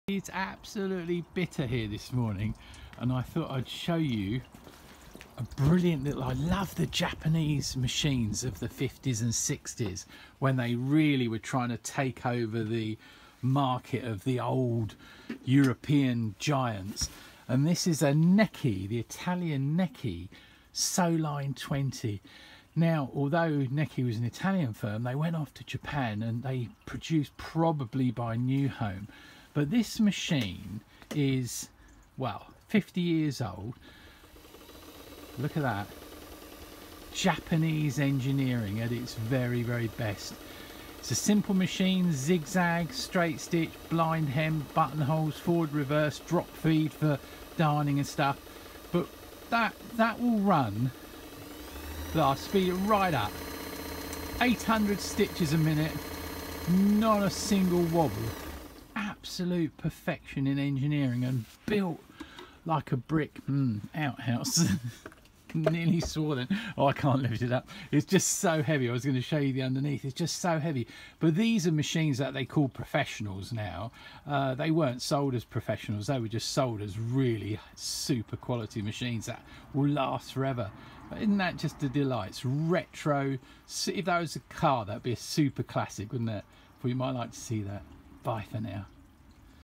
Alex Askaroff presents a perfect Necchi sewing machine in action.
Don't forget my books they make great presents,

Necchi made some superb machines. Here is one of their Japanese sewing machines from the 1980's, as heavy as a baby elephant but gloriously beautiful in performance. I hope you enjoy it.